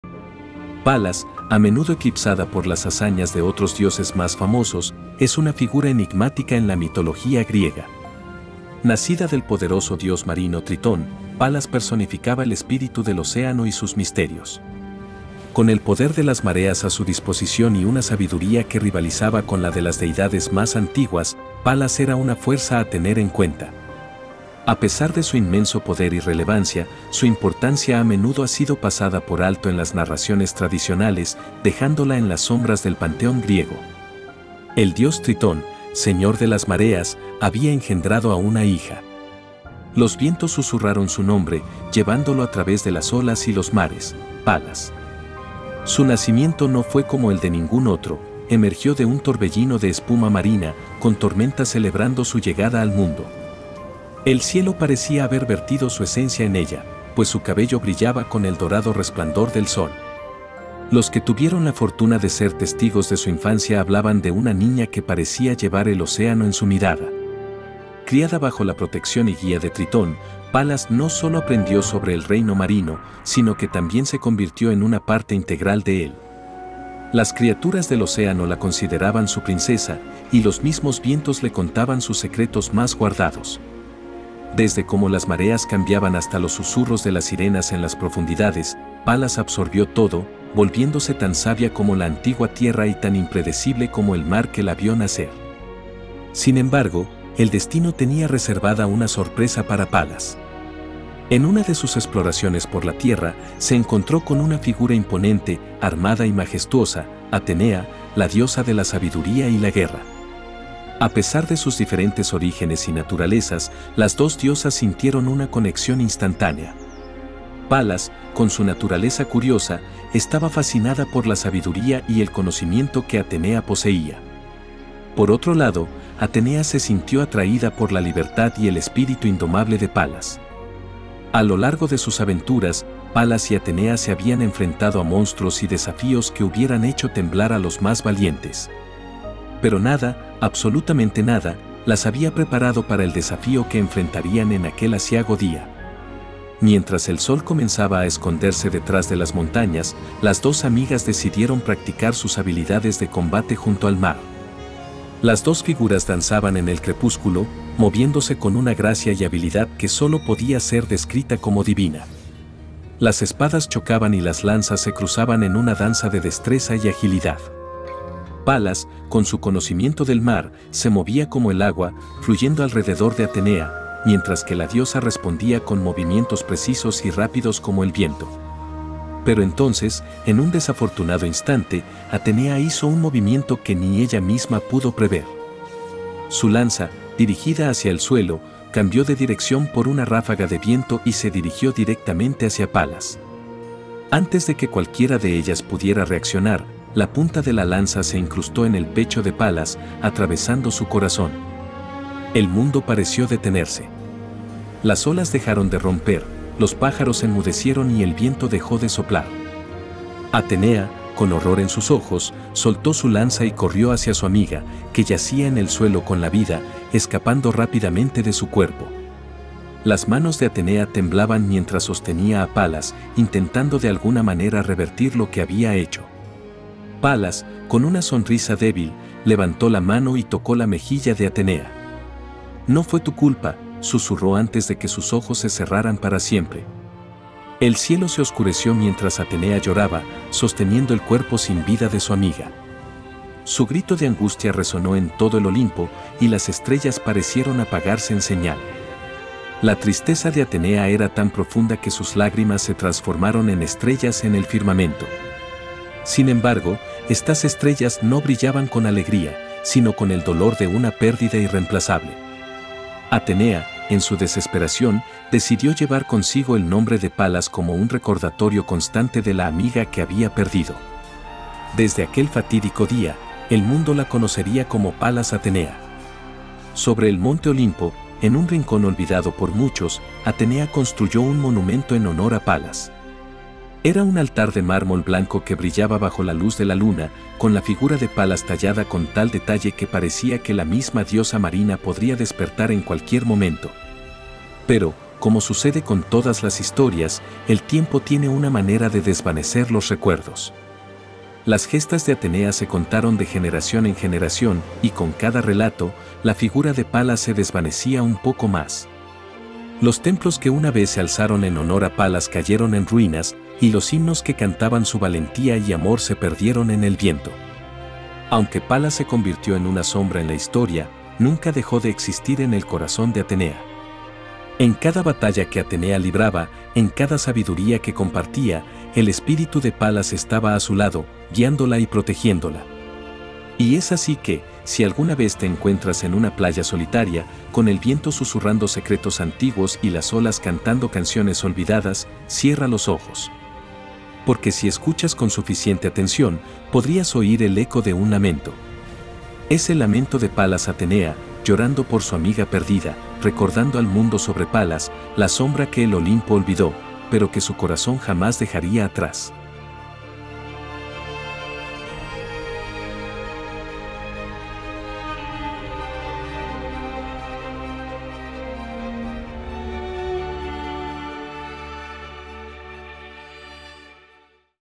Atenea: Cómo la Tragedia con Palas Definió a la Diosa de la Sabiduría
La Gran relación entre Atenea, la diosa griega de la sabiduría, y Palas, su misteriosa compañera olvidada por el Olimpo.